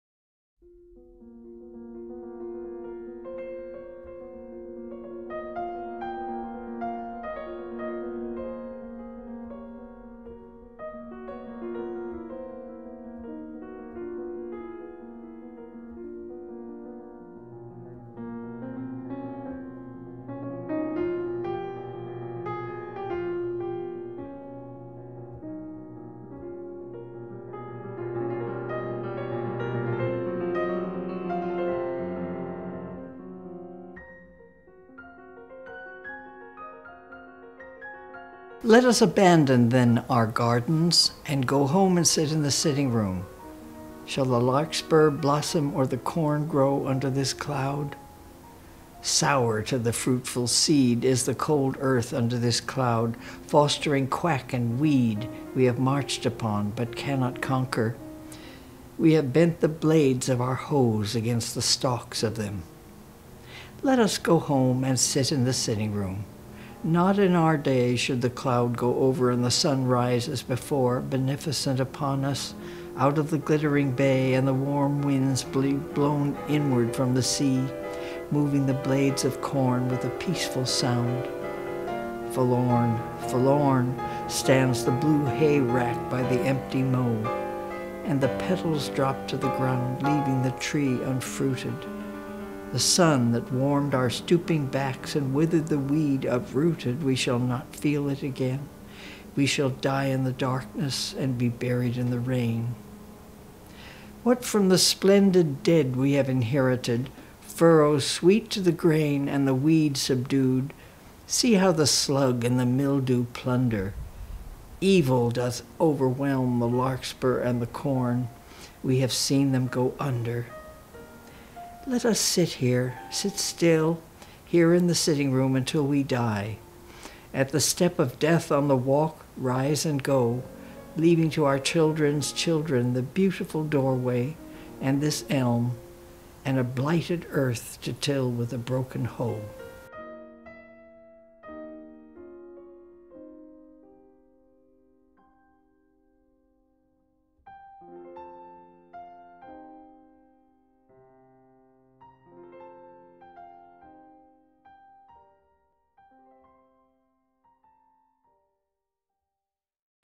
Justice Denied In Massachusetts - Short film adaptation
In 1947 modern dancer Judy Job composed and performed a dance to mark the 20th anniversary of the execution of Sacco & Vanzetti. Danced to the music of Ernest Bloch (here performed by Marian Conti, but in 1947 for Judy Job by the late pianist Naomi Sparrow) combined with a reading of Edna St Vincent Millay's poem, "Justice Denied in Massachusetts." With only two still photos from the 1947 performance surviving, this montage uses those two photos, as well as photos supplied by the Sacco & Vanzetti Commemoration Society in Boston (see link at the end of the video.)